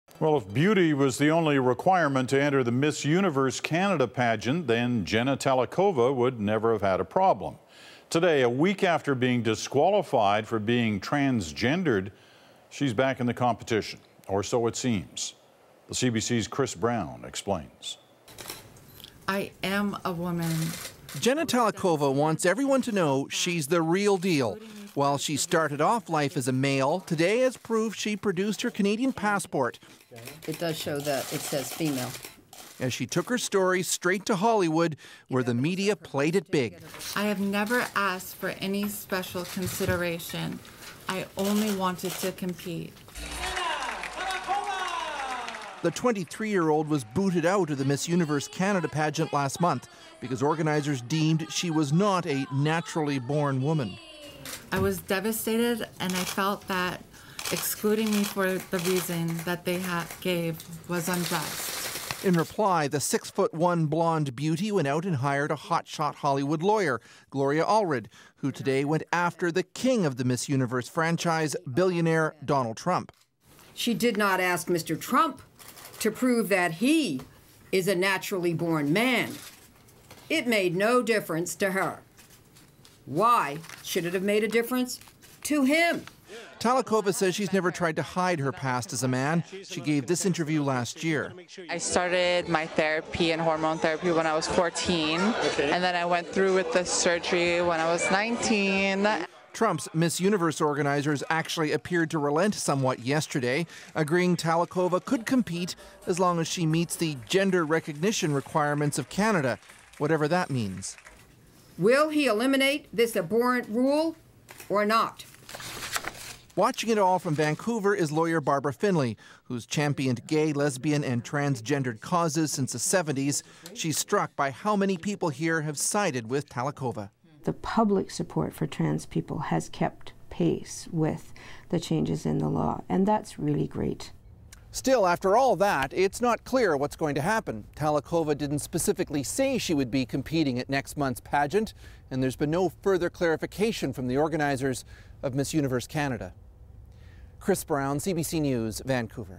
She's the Real Deal
If beauty was the only requirement to enter the Miss Universe Canada pageant, Jenna Talackova wouldn't have had a problem. Today, a week after being disqualified for being transgender she back in the competition.